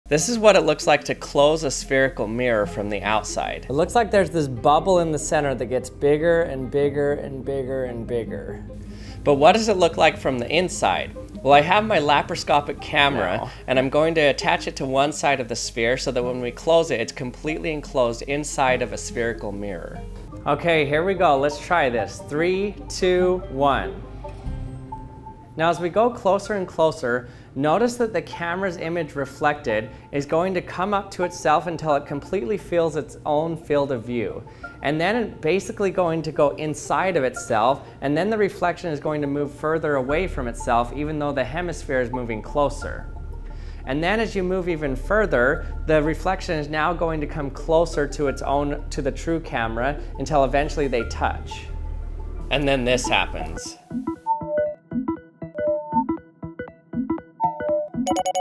What Does It Look Like Inside a Spherical Mirror?
I go inside a spherical mirror to exit the simulation...